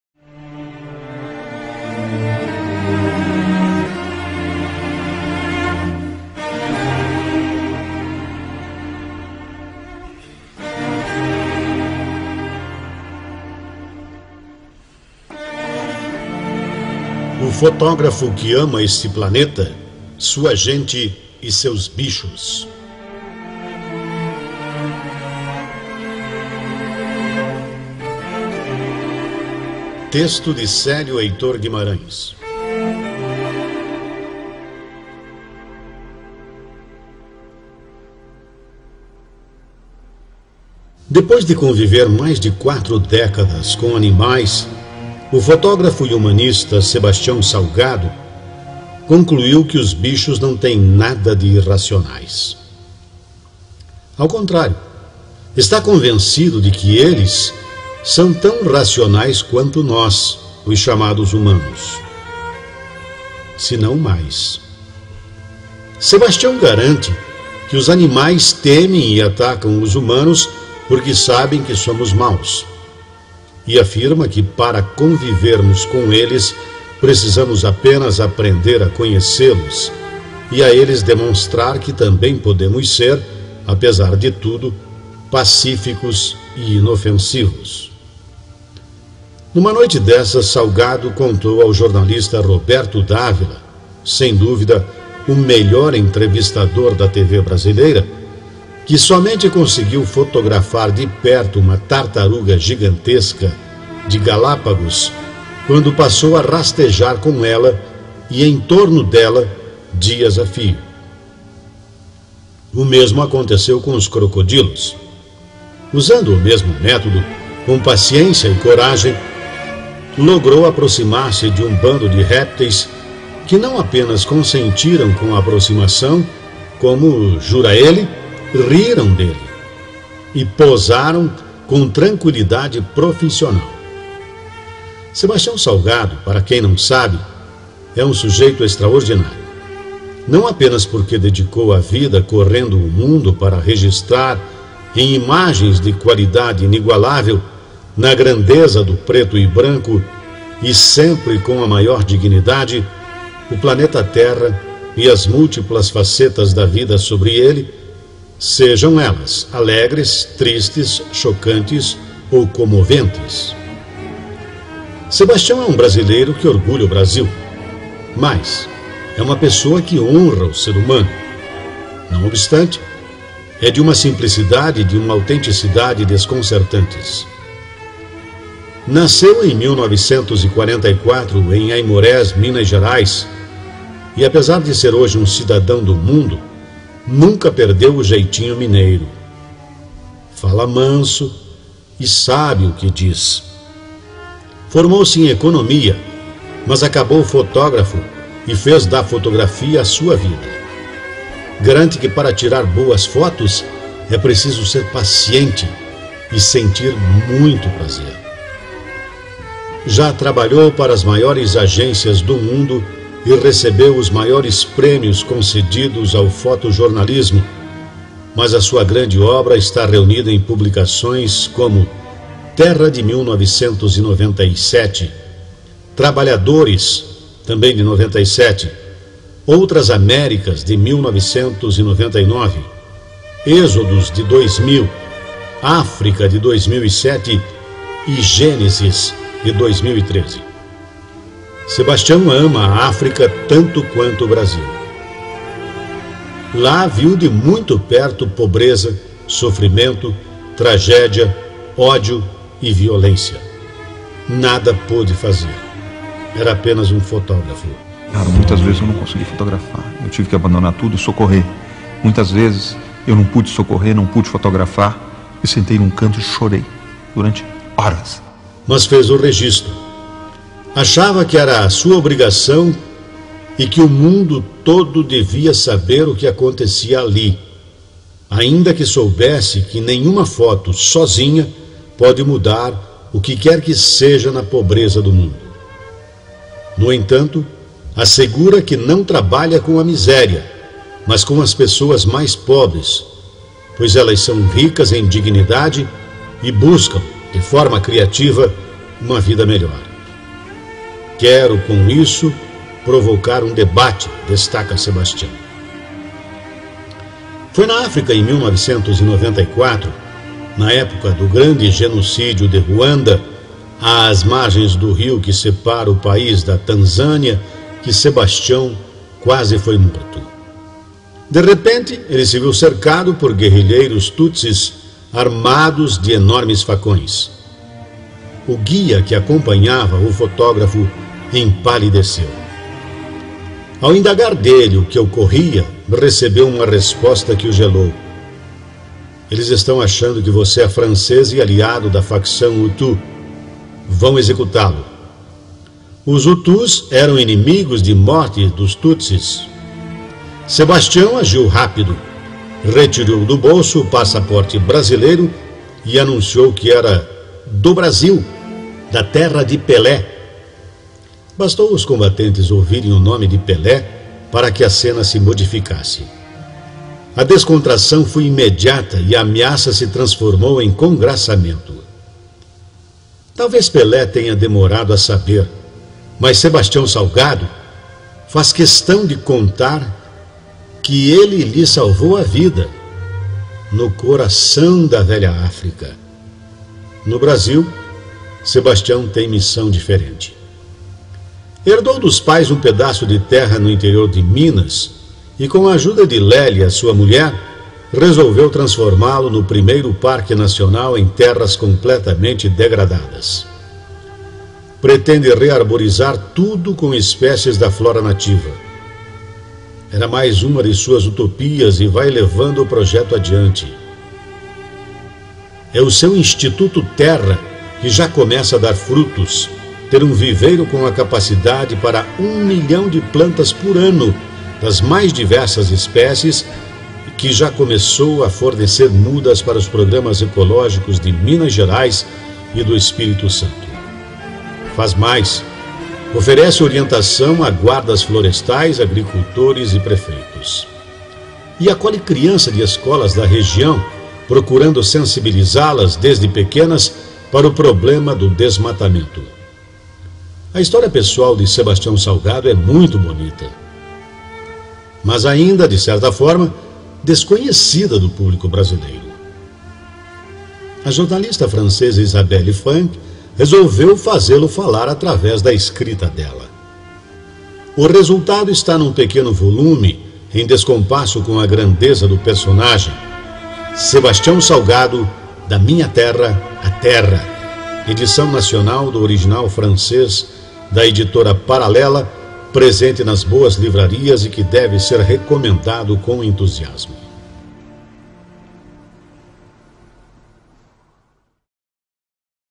SEBASTIÃO SALGADO
Um texto de Célio Heitor Guimarães, narração de Osni Gomes, em homenagem ao grande repórter fotográfico que é Sebastião Salgado e sua obra.